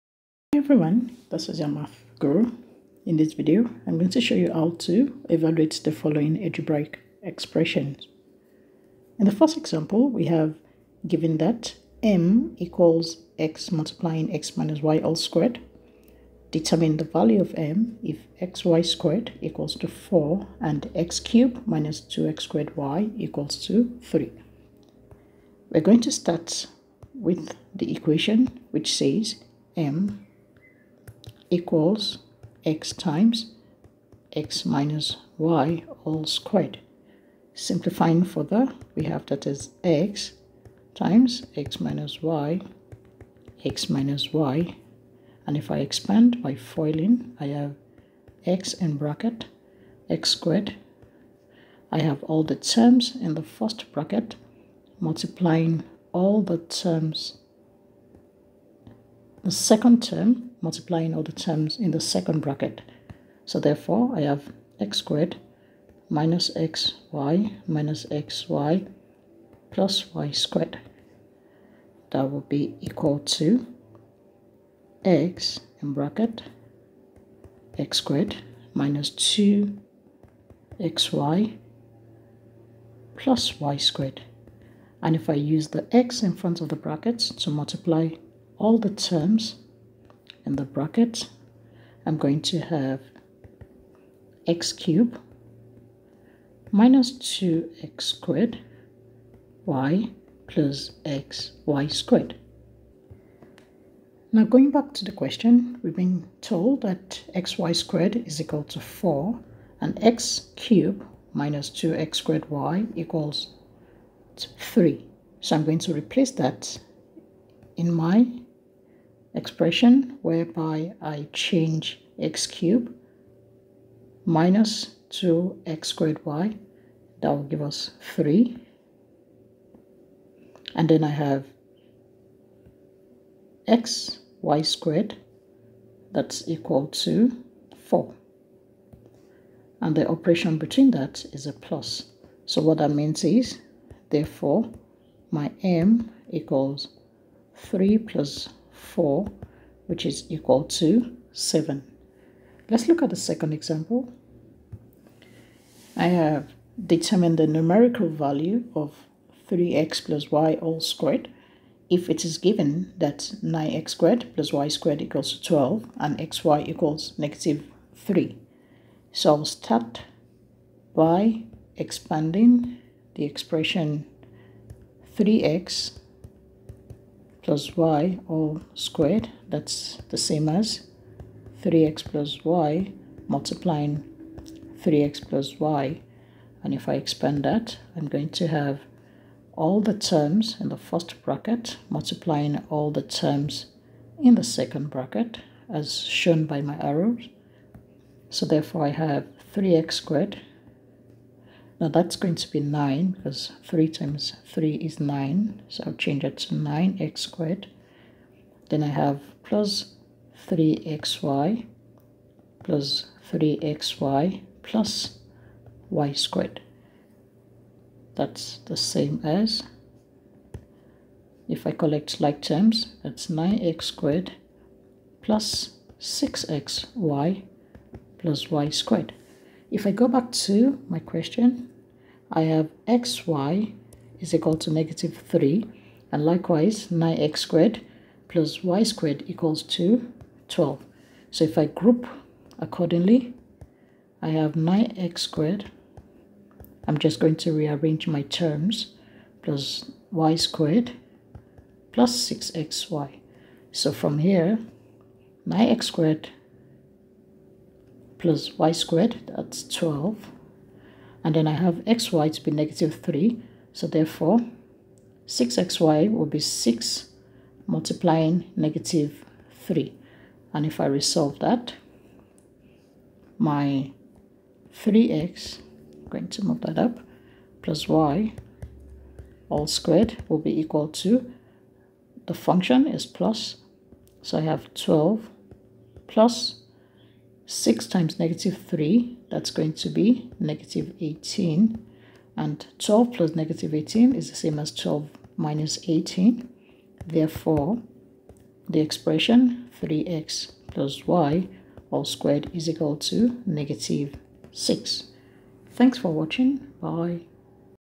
Algebraic Evaluation | Grade 10 | Example 4
This video teaches you how to simplify an expression to solve a problem.

Skills needed: Algebra, Simplification of algebraicexpression